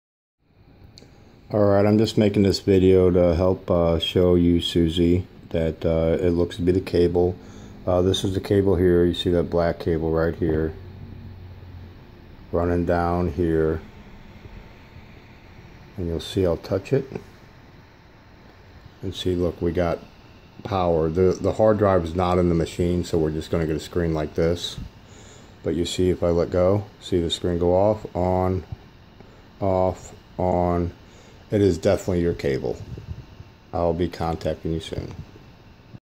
Diagnosing bad lcd cable vs. bad lcd screen
Short video: The laptop screen flickers and goes off and on from folding the laptop's lid. This shows me diagnosing it as a bad lcd cable vs. bad lcd. Sometimes similar symptoms occur from a bad lcd.